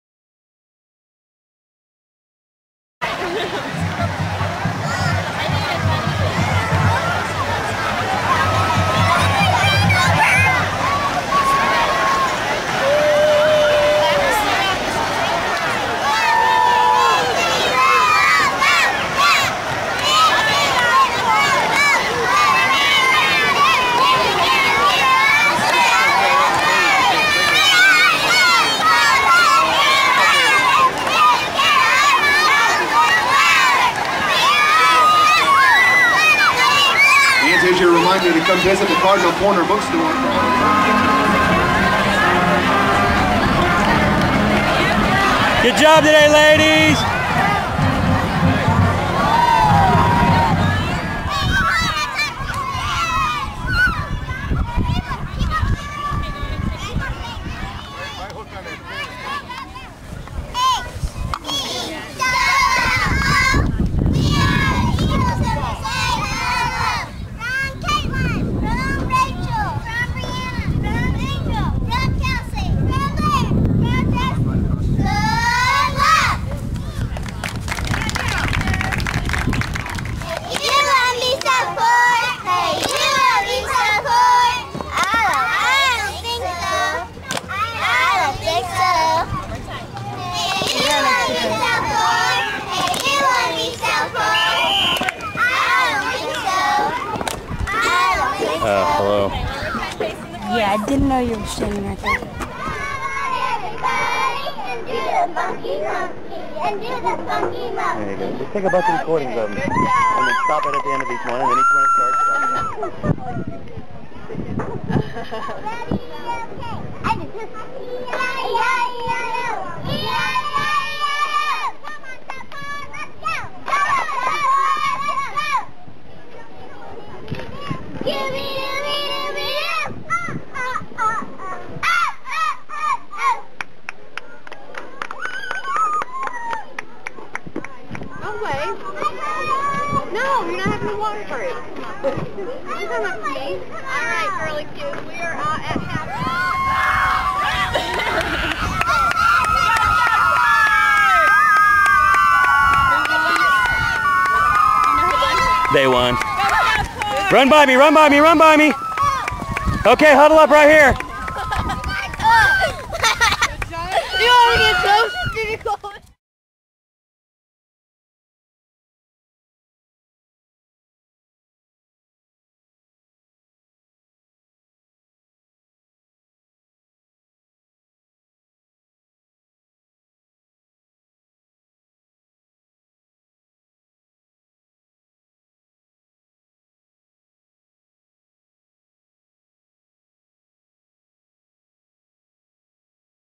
PTYFC and Game 2
Action from PTYFC Youth night and week 2 football game.